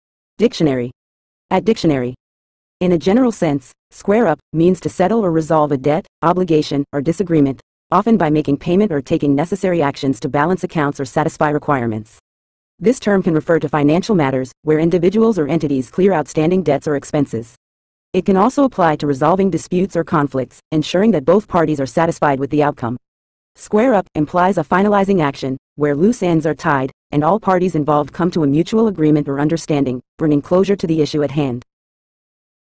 Square up Meaning In English
In a general sense, "square up" means to settle or resolve a debt, obligation, or disagreement, often by making payment or taking necessary actions to balance accounts or satisfy requirements. This term can refer to financial matters, where individuals or entities clear outstanding debts or expenses. It can also apply to resolving disputes or conflicts, ensuring that both parties are satisfied with the outcome. "Square up" implies a finalizing action, where loose ends are tied, and all parties involved come to a mutual agreement or understanding, bringing closure to the issue at hand.